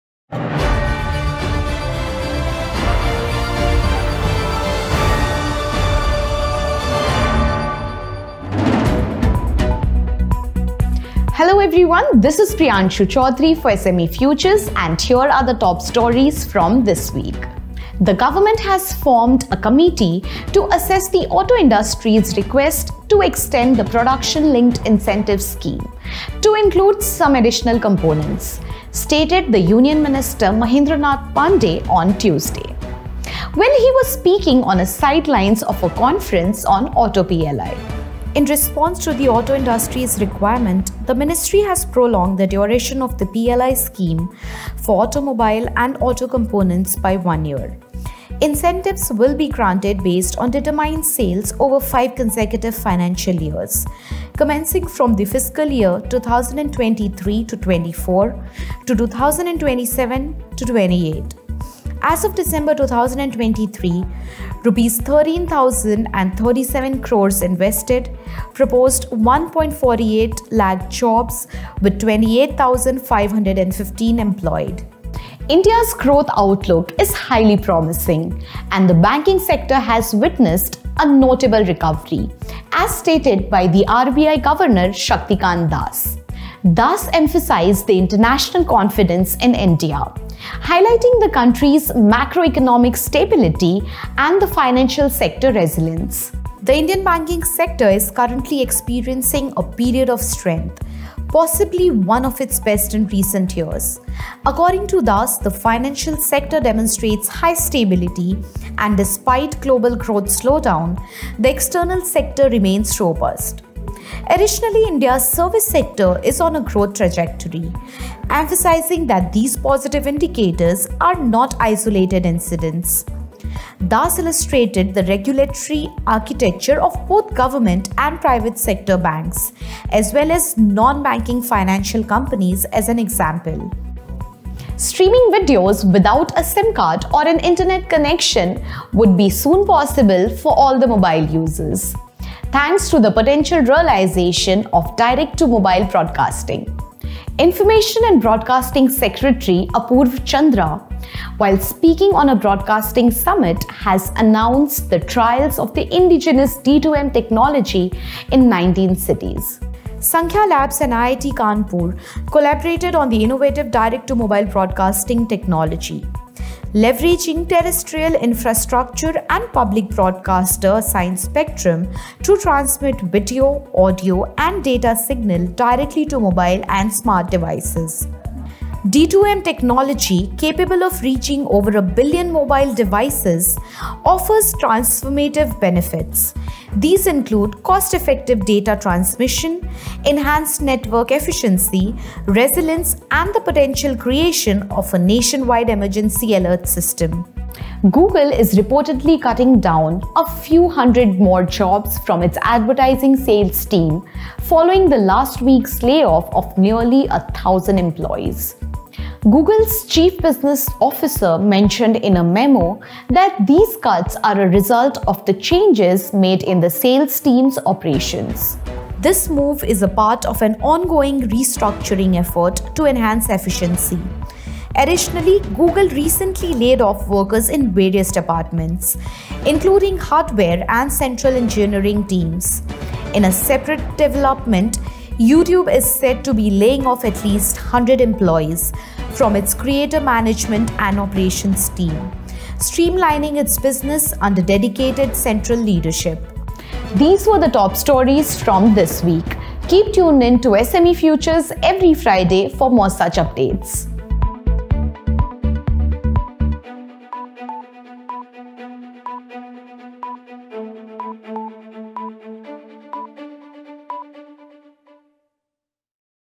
Auto Sector's Plea: Expand PLI Components
Government panel to review Auto PLI component demands, RBI chief expresses optimism about India's growth prospects, Direct-to-mobile broadcasting trials set for 19 cities: I&B secretary, Google, YouTube announce job cuts